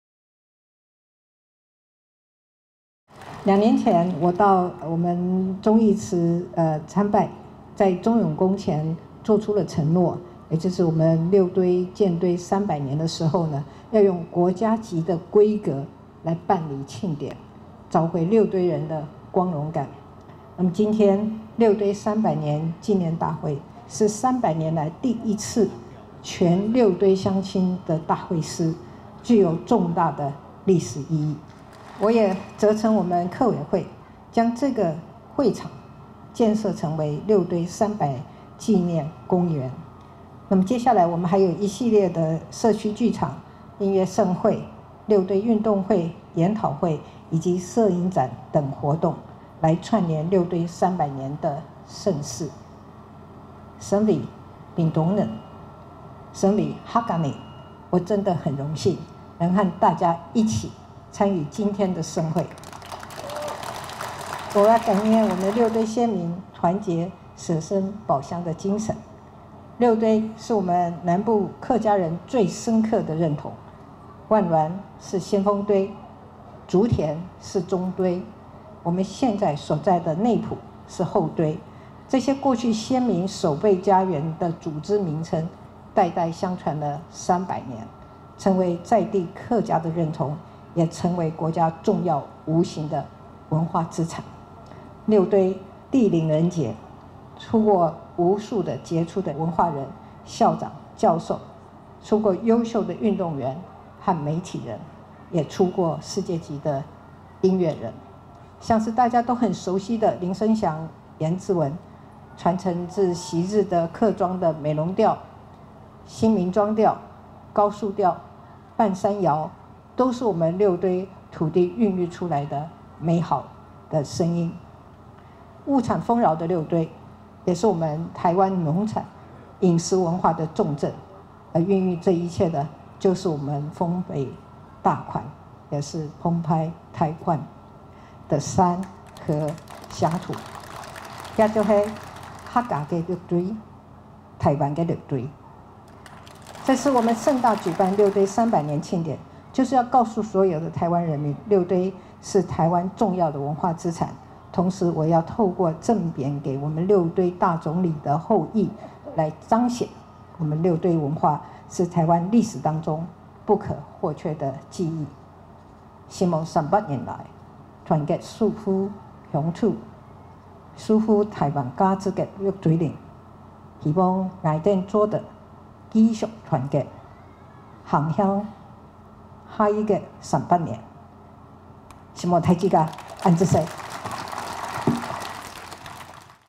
20211120 總統出席「六堆300紀念大會」
蔡英文總統前往屏東出席「六堆300紀念大會」，與六堆客家鄉親一同感念六堆先民團結、捨身保鄉的精神；她也強調，物產豐饒的六堆不僅是臺灣農產、飲食文化的重鎮，更是臺灣重要的文化資產。「這就是客家的六堆、臺灣的六堆」。